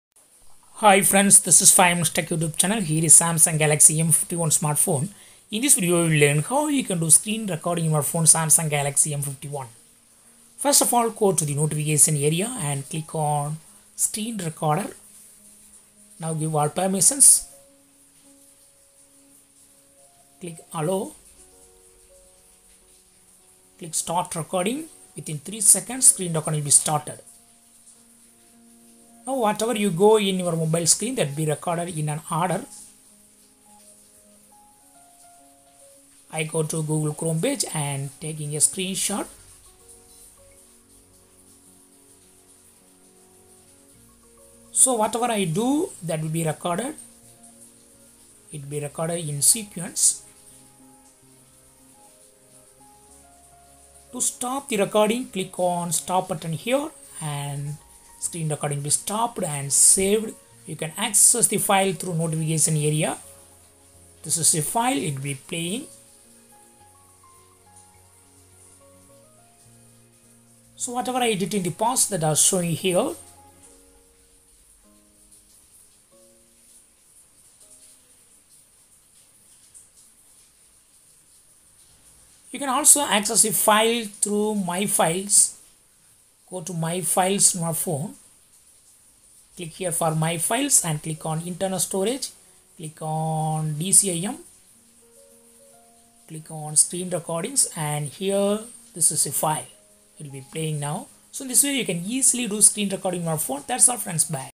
How to do screen recording in Samsung Galaxy M51
Samsung Galaxy M51 screen recording settings: Learn here how to do screen recording in Samsung Galaxy M51 smartphone